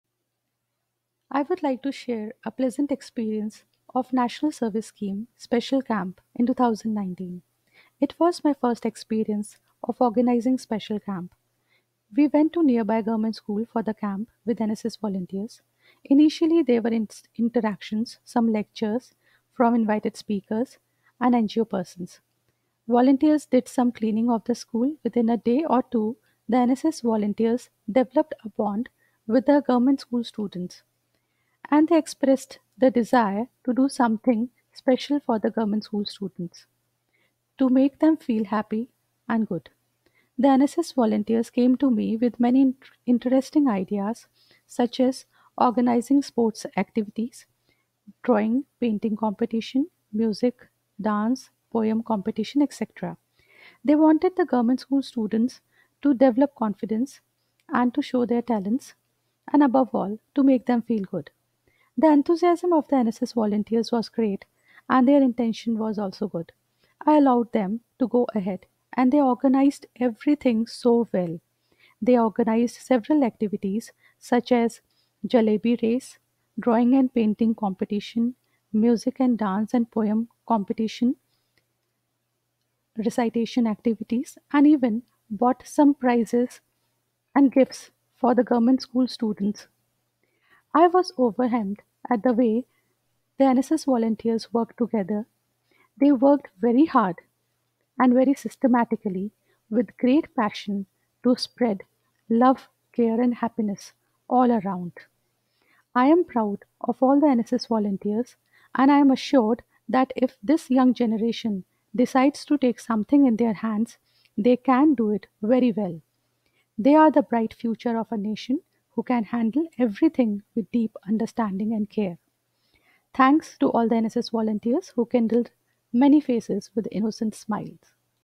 Bringing smiles on innocent faces-NSS, University of Kota
National Service Scheme volunteers spreading love and happiness